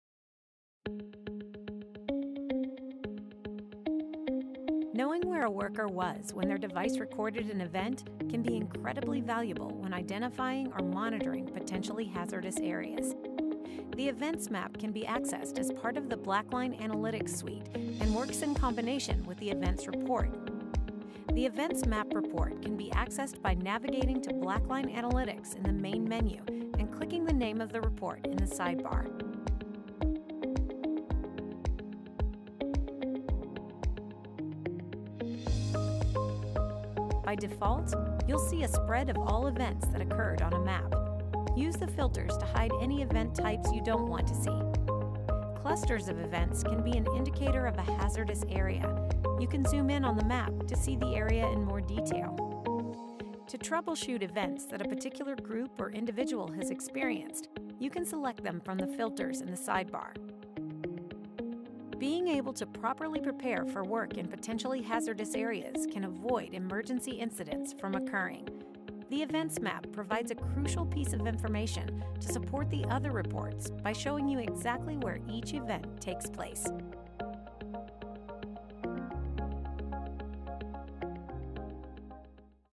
Events Map Report
This video outlines how to use the Events Map report in Blackline Analytics. Need help with using Blackline Analytics?